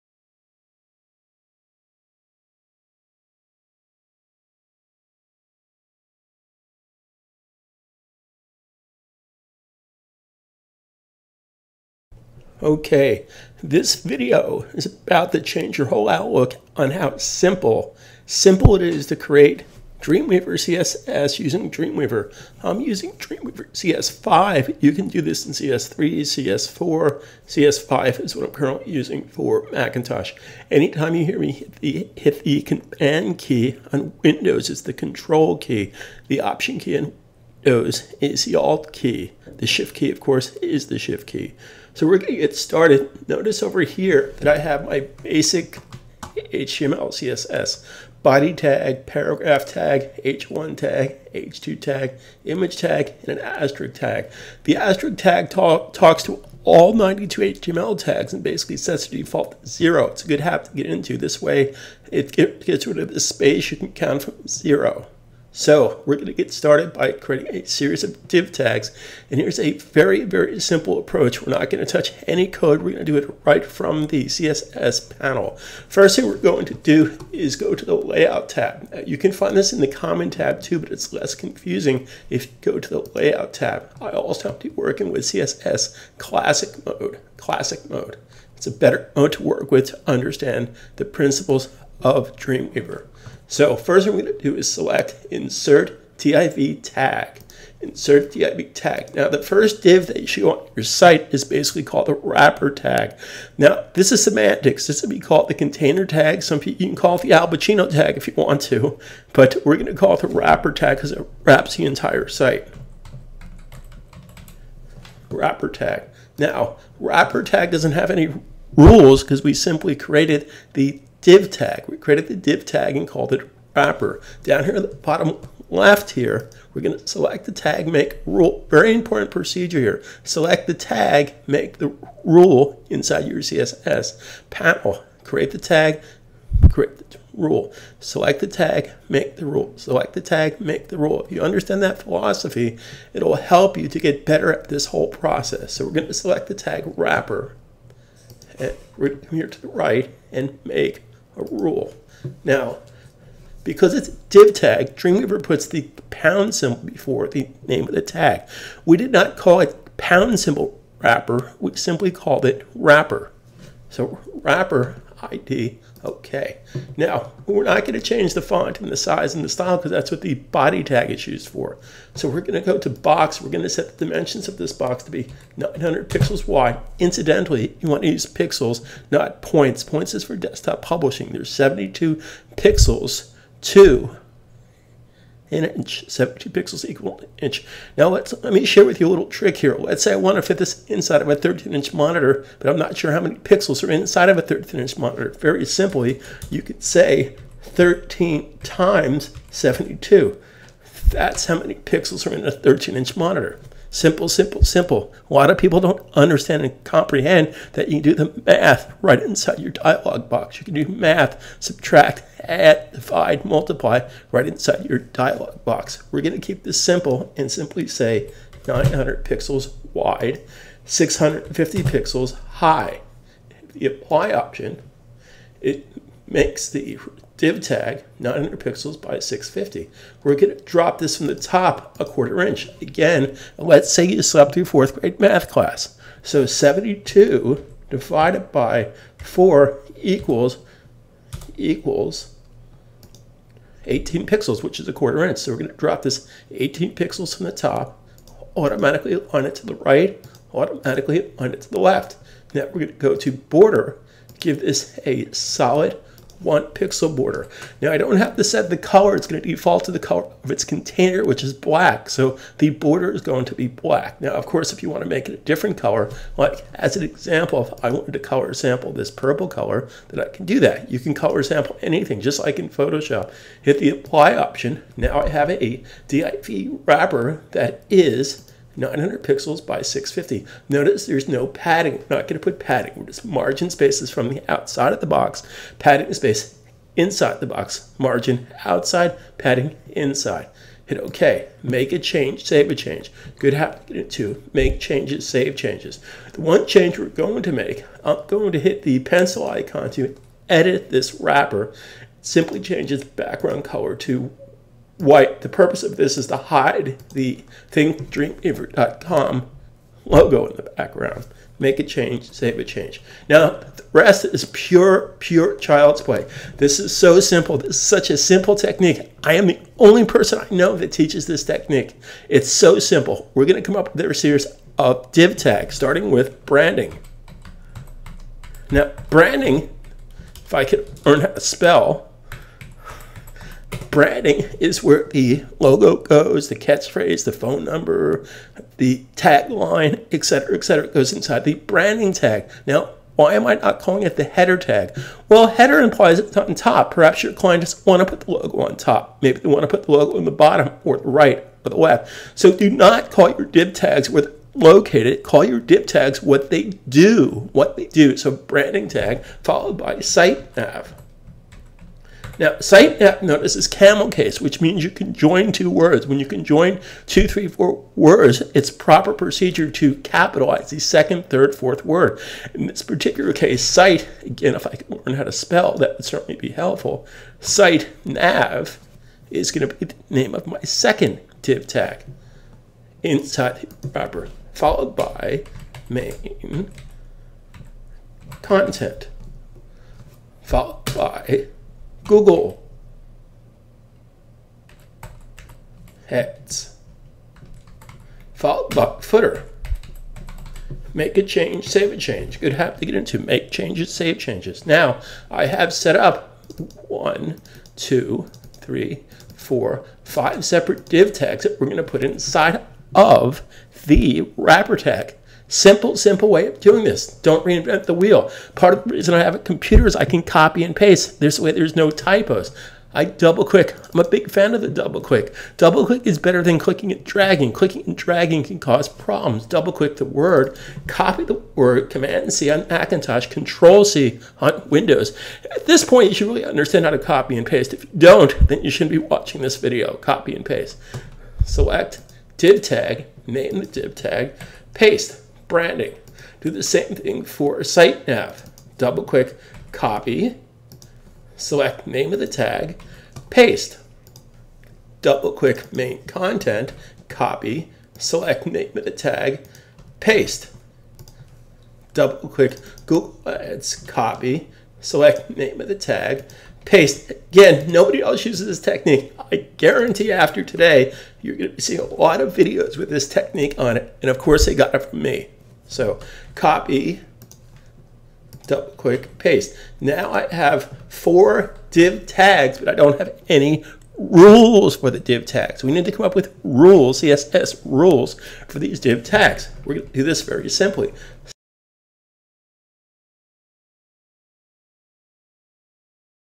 Learn how to design build an CSS website web page with div tags Dreamweaver CS5 tutorials part 1
A very simple approach to build a professional website the right way using Dreamweaver CSS build your css site without knowing html code - learn more go taught by master adobe teacher since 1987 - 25 years html5 instructional video tutorial